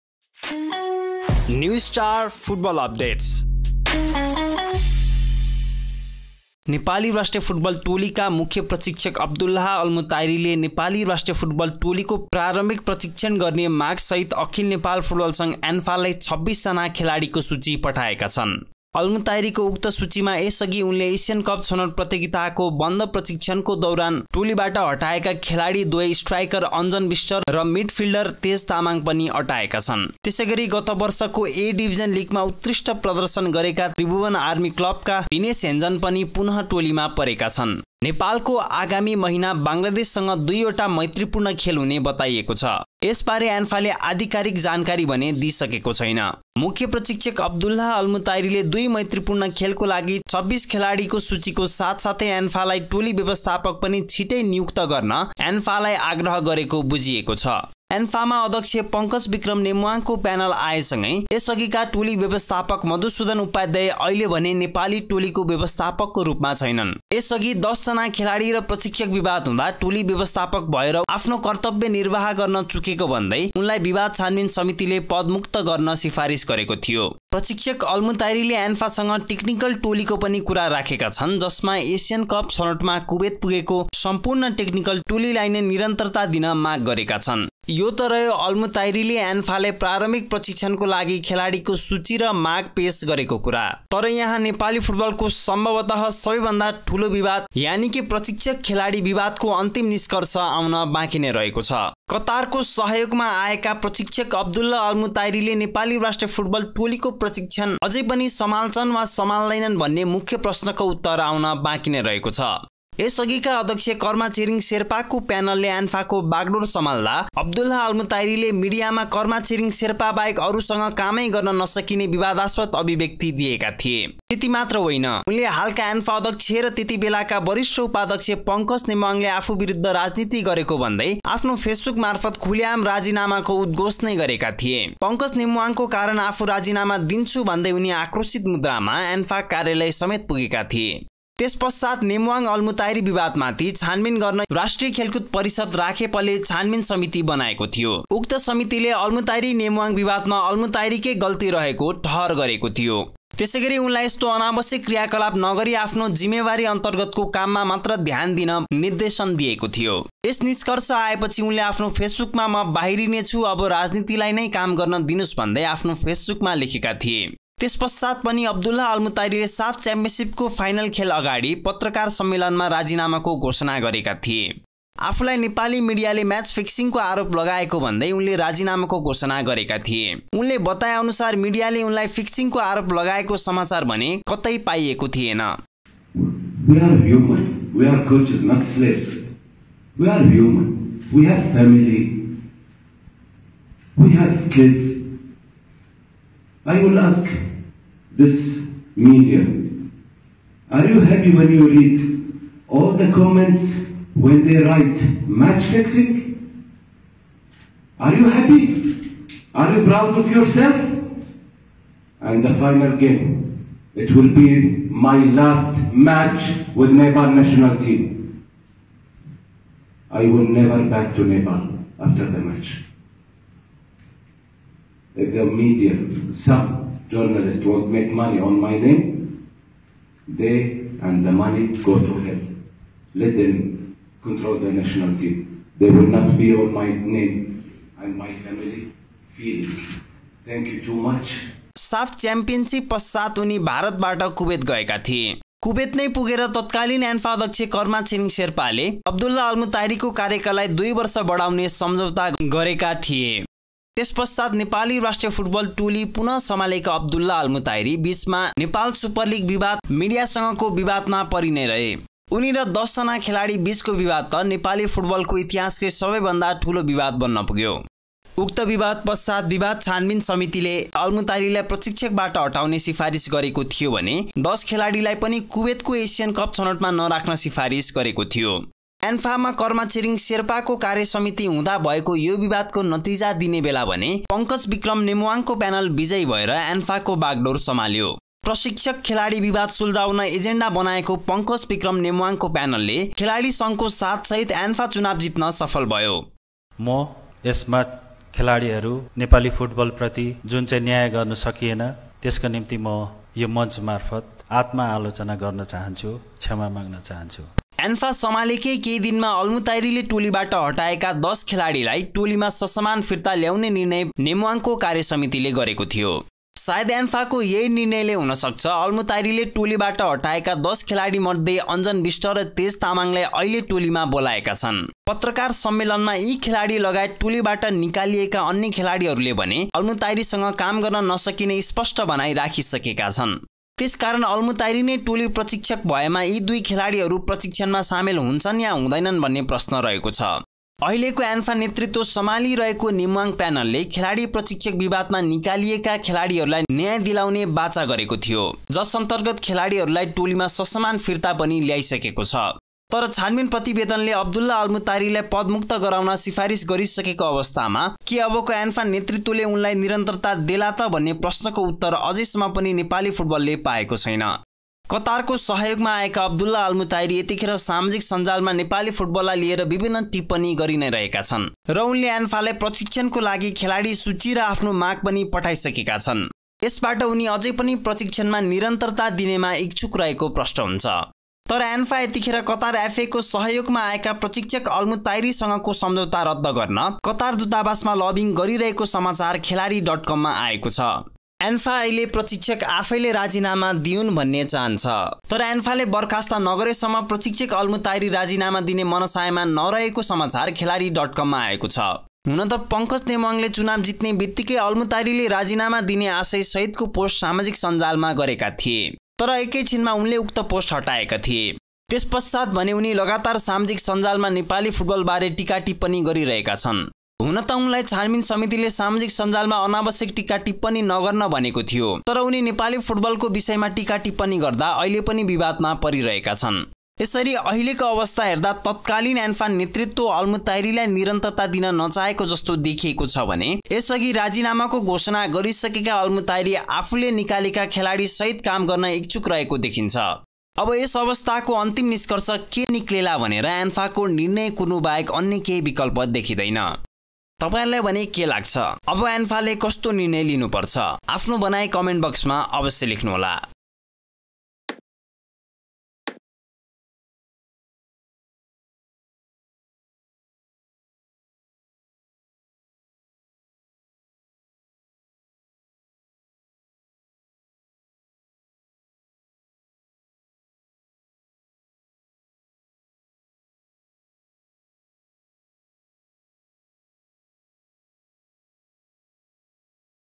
के एन्फाले अल्मुताइरीलाई निरन्तरता देला ?Abdullah Almutairi controversies, ANFA and Nepali Football
नेपाली फुट्बलको सम्भवत : सबै भन्दा ठुलो विवाद, प्रशिक्षक - खेलाडी विवादको अन्तिम निश्कर्ष आउन बांकी नै रहेको छ । कतारको सहयोगमा आएका प्रशिक्षक अबुल्लाह अल्मुताइरीले नेपाली राष्ट्रिय फुटबल टोलीको प्रशिक्षकमा निरन्तरता पाउँछन् वा पाउँदैनन् भन्ने विषयको टुंगो लाग्न बाँकी नै रहेको छ । यस अघिका अध्यक्ष कर्मा छिरिङ्ग शेर्पाको प्यानले एन्फाको बागडोर  सम्हाल्दा अल्मुताइरीले मिडियमा कर्मा छिरिङ्ग शेर्पा बाहेक अरु सँग काम गर्न नसक्ने विवादास्पद अभिव्यक्ती दिएका थिए । त्यती मात्र हैन उनले हालका एन्फा अध्यक्ष र त्यतिबेलाका वरिष्ट उपाध्यक्ष पंकज नेम्बाङ्गले आफू विरुद्द राजनीति गरेको भन्दै आफ्नो फेसबुक मार्फत खुलेआम उद्घोष नै गरेका थिए । पंकज नेम्बाङ्गको कारण आफू राजीनामा दिन्छु भन्दै उनी आक्रोशित मुद्रामा एन्फा कार्यलय समेत पुगेका थिए । त्यसपश्चात  नेम्वाङ–अल्मुताइरी विवादमाथि छानविन गर्न युवा तथा खेलकुद मन्त्रालयले राष्ट्रिय खेलकुद परिषद (राखेप)का तत्कालीन सदस्य पुरेन्द्रविक्रम लाखेको संयोजकत्वमा छानविन समिति बनाएको थियो ।
उक्त समितिले अल्मुताइरी-नेम्वाङ्ग विवादमा अल्मुताइरी कै गल्ती रहेको ठहर गर्केओ थियो । त्यसैगरी उनलाई यस्तो अनावश्यक क्रियाकलाप नगरी आन्फो जिम्मेवारी अन्तर्गतको काममा मात्र ध्यान दिएको थियो । यस निष्कर्ष आए पछी उनले आफ्नो फेसबुकम म बाहिरिनिछु अब राजीनीतिलाई काम गर्न दिनुस भन्दै आफ्नो फेसबुकमा लेखेका थिए  द पोलिटिक्स वर्क...’ लेखेका थिए । त्यसपश्चात पनि उनले साफ च्याम्पियनसिपको फाइनल खेल अगाडी पत्रकार सम्मेलनमा राजीनामाको घोष्णा नै गरेका थिए । आफूलाई नेपाली मिडियाले  म्याच फिक्सिङ्गको आरोप लगाएको भन्दै उनले राजीनामाको घोषणा गरेका   ।  उनले बताए अनुसार मिडियाले उनलाई फिक्सिङ्गको आरोप लगाएको समाचार भने पाइएको थिएन । साफ च्याम्पियनसिप पश्चात उनी कुवतै गएका थिए र कुवतनै पुगेर तत्काअलिन एन्फा अध्य्कक्ष कर्मा छिरिङ्गग सेर्पाले उनको कार्यकाल २ वर्ष निरन्तरता दिने सम्झौता भएको थियो । त्यसपश्चात नेपाली टोली पुन : सम्हालेका अल्मुताइरी बिचमा एनएसल, मिडिया सँगको अन्तर्कृया लगायत विवाद पा परी नै रहे । उनी र १० खेलाडी विवाद त नेपाली फुट्बल ईतिहासकै ठुलो विवाद बन्न पुग्यो ।

के अल्मुताइरीलाई एन्फाल निरन्तरता देला ? Abdullah Almutairi controversies, ANFA and Nepali Football

* Uploading this video is Strictly Prohibited.
* Using the clips of the video without permmison is not allowed.
* Embedding the video link is allowed